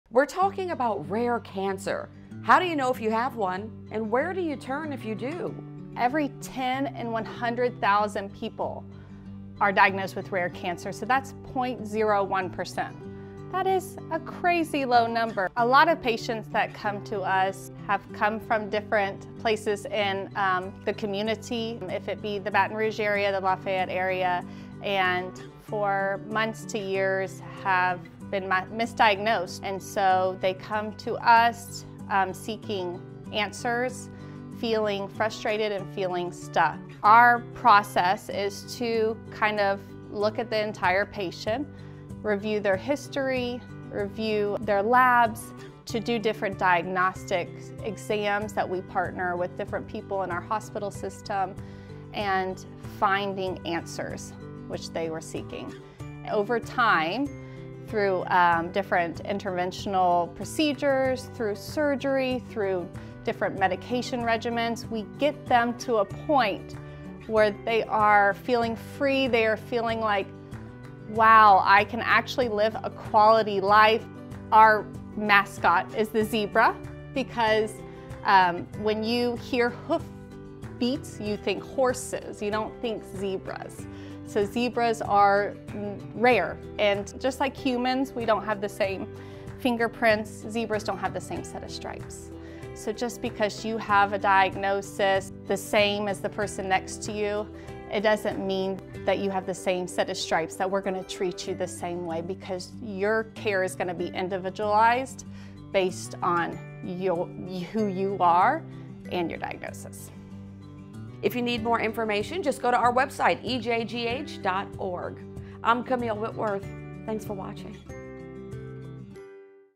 Healthy Lifestyles | Rare Cancer Treatment & Program
🎗 On this HealthyLifestyles we're talking about rare cancer. How do you know you have one, and where do you turn if you do? Sierra Vilmar, APRN from Rare Cancer Treatment & Program helps us navigate through this rare conversation.